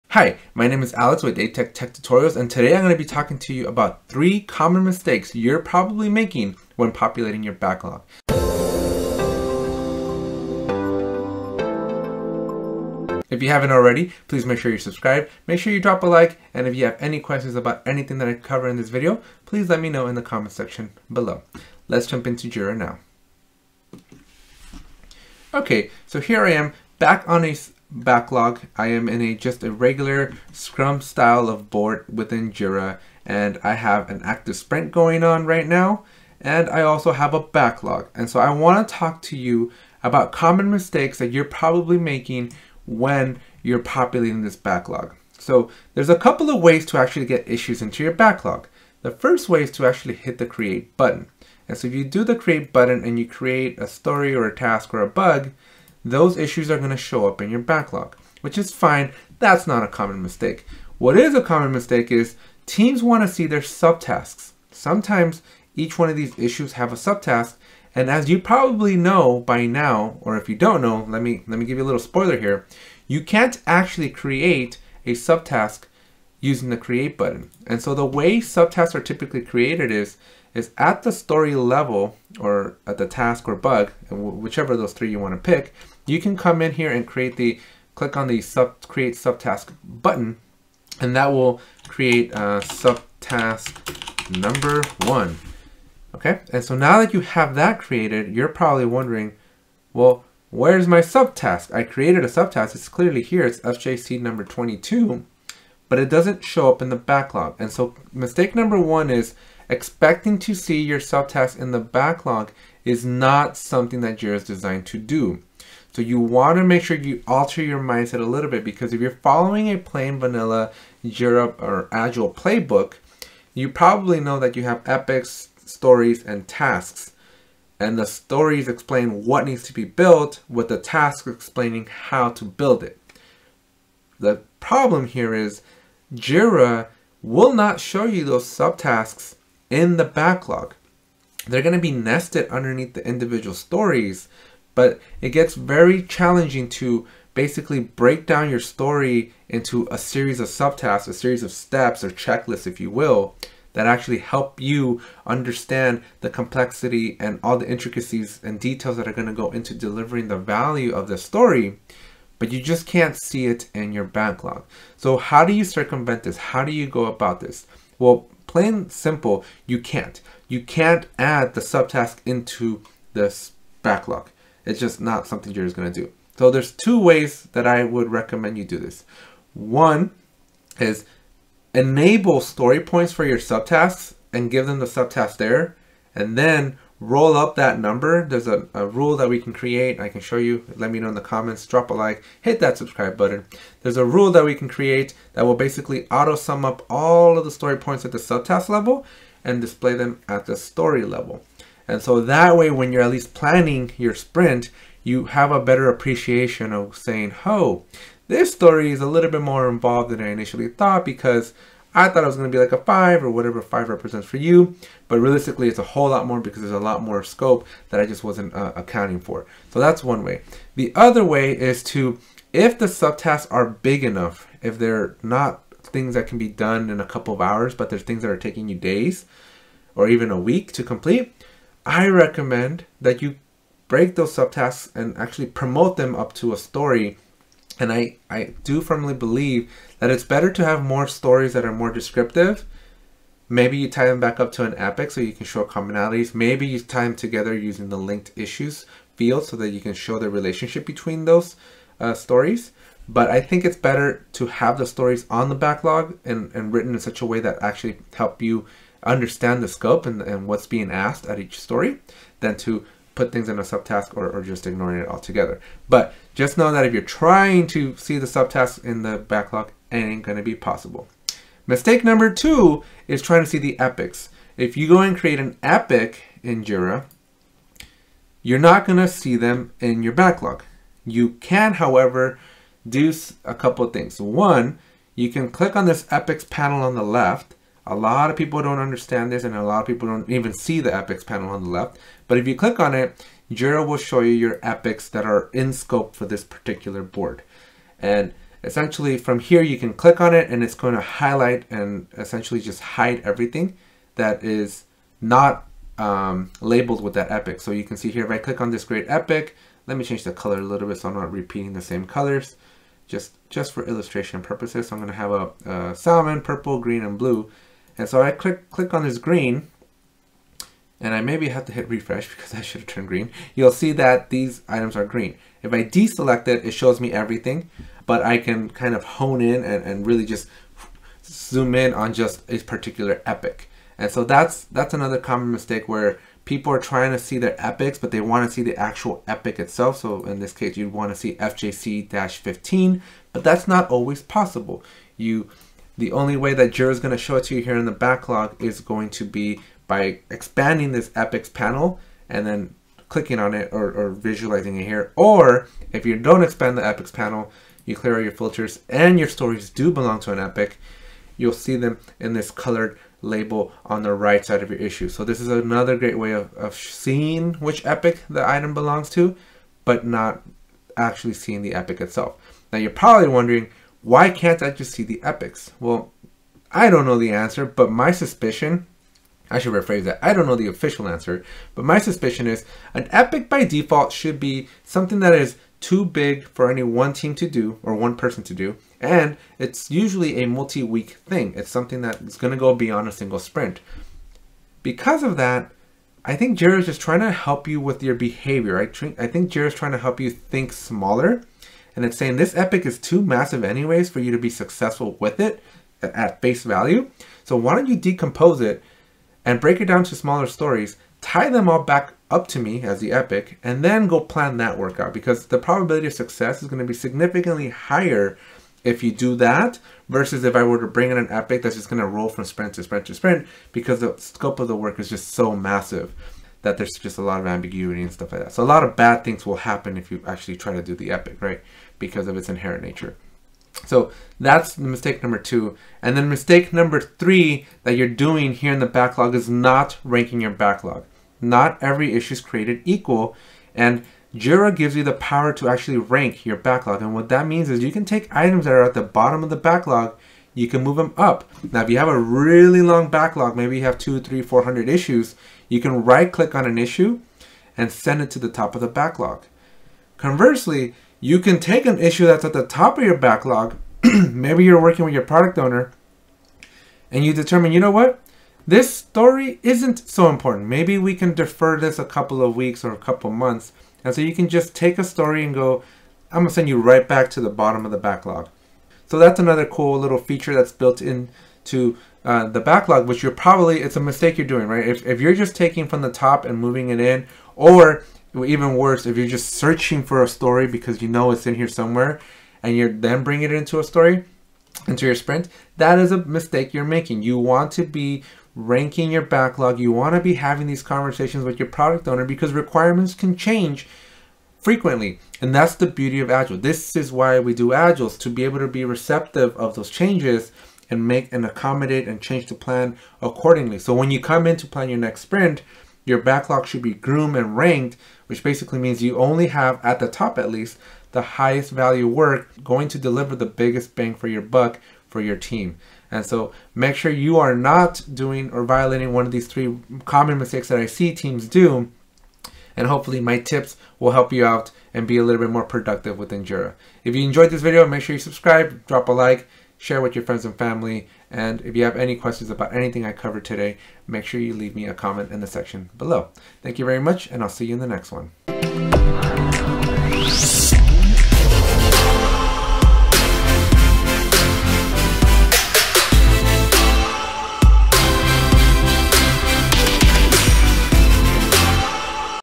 3 Common Backlog Mistakes Scrum Masters Make | Atlassian Jira
In this Atlassian Jira video you are going to learn about 3 common mistakes that many teams make when populating a backlog. Populating backlogs can sometimes be tricky, watch this video to avoid doing the same mistakes.

Mistake Number 1: Wanting to see sub-tasks in your Scrum backlog.

Mistake Number 2: Expecting to see Epics in the backlog without expanding the Epics panel

Mistake Number 3: Not ranking your backlog